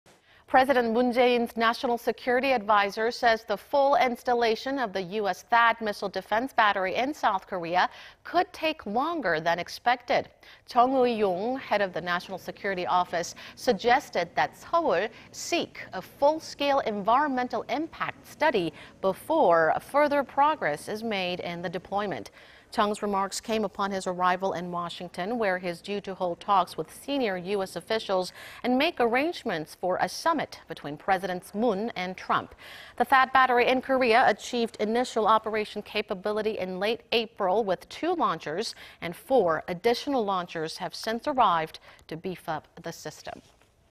President Moon's national security adviser says S. Korea to assess environmental impact of THAAD
정의용 "사드 환경영향평가 철저히…배치 시간 더 걸릴 듯"

  President Moon Jae-in's national security adviser says the full installation of the U.S. THAAD missile defense battery in South Korea could take longer than expected.
  Chung Eui-yong  , head of the National Security Office, suggested that Seoul seek a full-scale environmental impact study, before further progress is made in the deployment.
Chung's remarks came upon his arrival in Washington, where he's due to hold talks with senior U.S. officials, and make arrangements for a summit between Presidents Moon and Trump.
The THAAD battery in Korea achieved initial operational capability in late April with two launchers, and four additional launchers have since arrived to beef up the system.